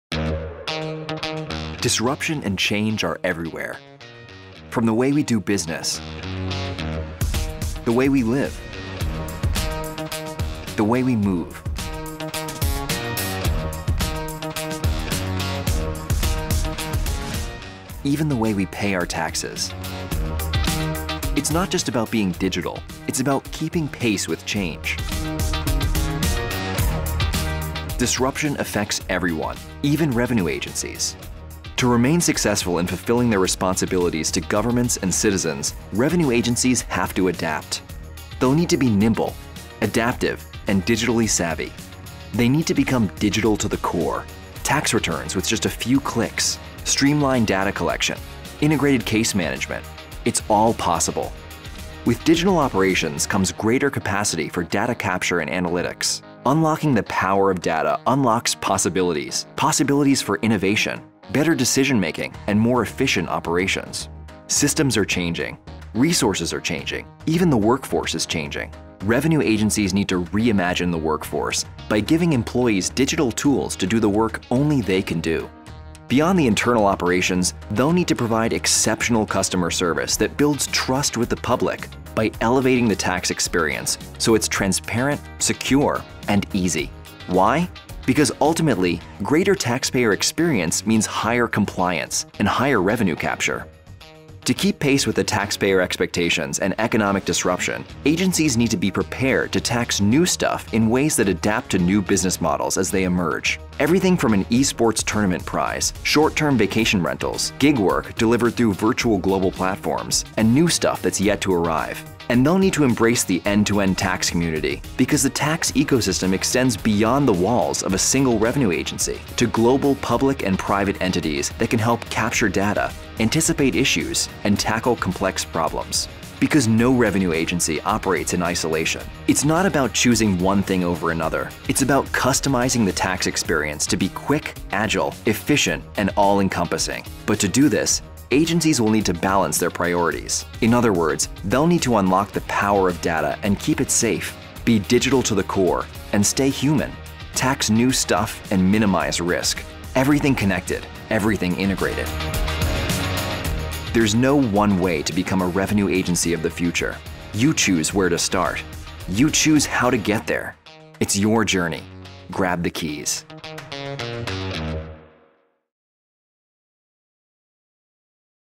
The revenue agency of the future: Seven keys to digital transformation
In a world of accelerating digital and economic disruption, many revenue agencies recognize that yesterday’s taxation models can no longer keep pace with the technology revolution reshaping our world. But while many leaders understand the importance of taking digital transformation seriously, few have cracked the code to unlock a long-term, “future-proof” model for mission success.

So what can revenue agencies do to successfully keep pace with rapid technological change and evolving citizen expectations? In this piece, we explore seven keys that can guide their journey to digital transformation. Read the report